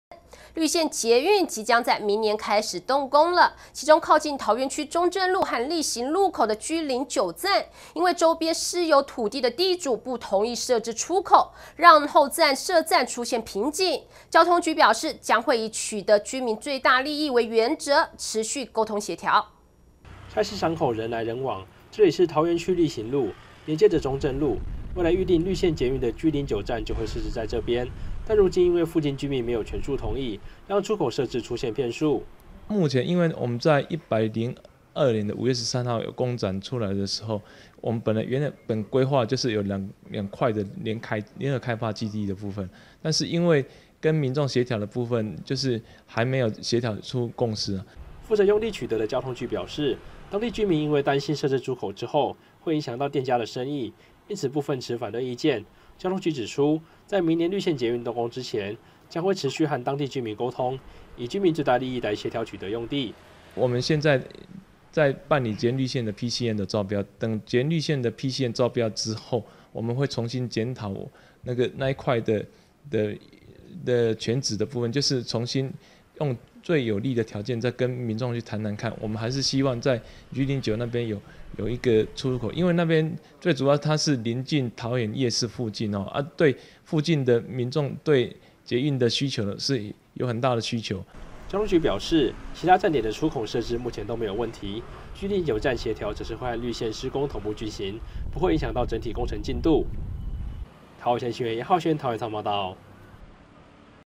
桃園有線新聞20161010-捷運綠線G09站卡關 出口用地待協調
綠線捷運即將在明年開始動工，其中靠近桃園區中正路力行路口的G09站，因為周邊私有地地主不同意設置出口，讓後續設站出現瓶頸，交通局表示將會以取得居民最大利益為原則持續溝通協調。